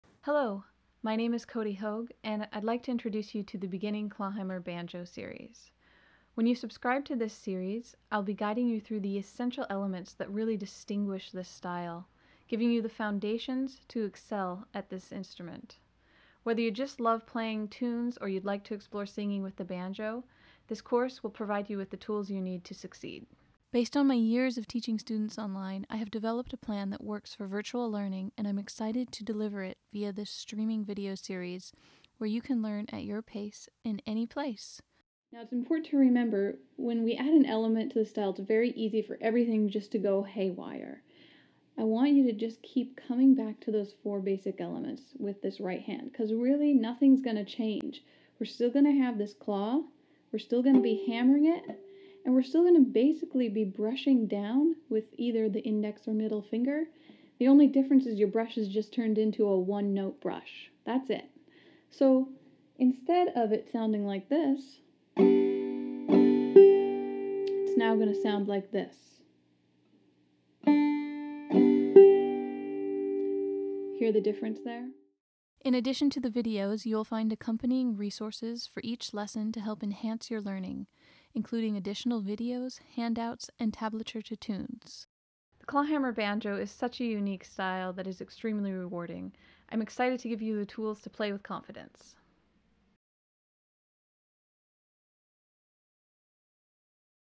Beginning Clawhammer Banjo Lesson Series Info Video
The Beginning Clawhammer Banjo Lesson Series is a streaming video lesson series that will guide you through the essentials of this the style. We'll start with mastering the bum-ditty rhythm pattern, then explore left-hand techniques, tunes, alternate tunings, and even and introduction to singing with the banjo. For just $25 a month (half the price of one private online lesson), you will have continuous access to the material as long as your subscription is active, and there are no restrictions - you can cancel at any time.